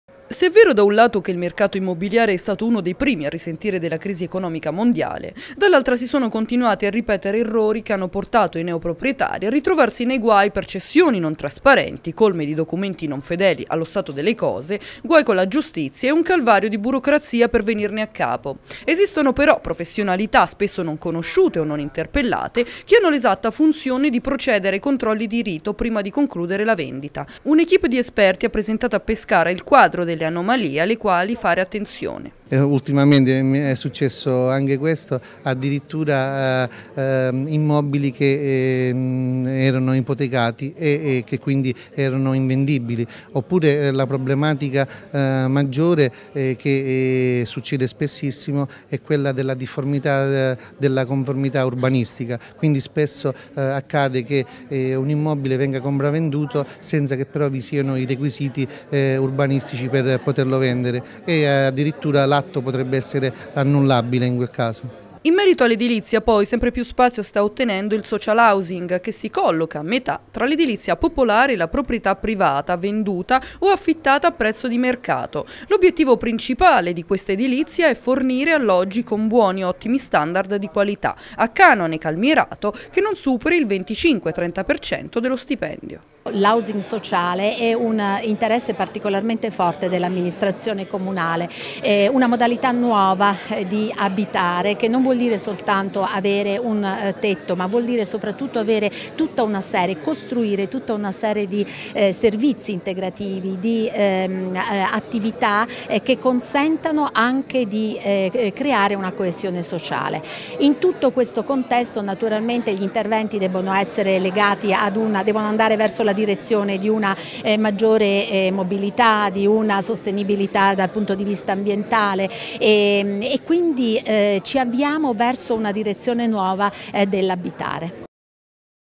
Mercato immobiliare. Gli esperti mettono in guardia
Il mercato immobiliare tra crisi economica e guai burocratici. Tante le difficoltà e gli errori nei quali incappano spesso gli acquirenti. A Pescara, nel corso di un convegno, un pool di esperti ha messo in guardia sugli errori più comuni